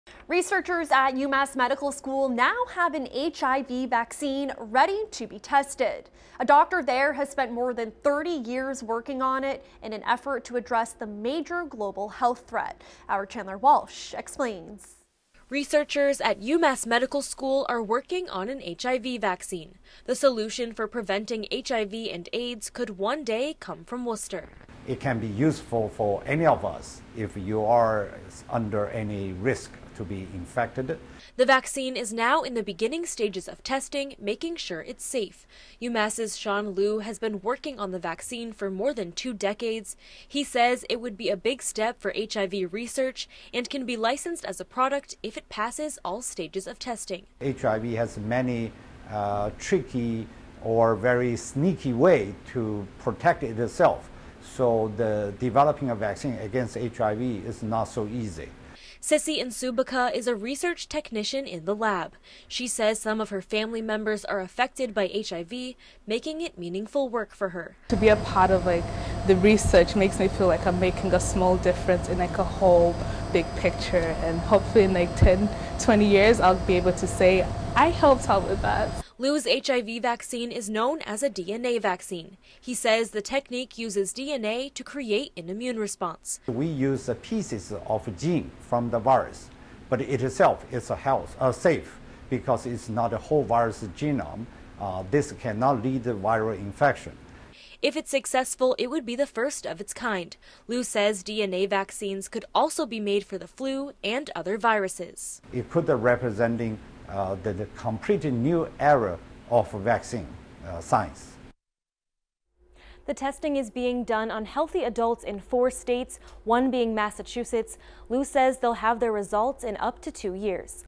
WNT UMass HIV Vaccine - March 27th, 2018
Chandler Walsh reports- Researchers at UMass Medical School could be one step closer to preventing the HIV infection.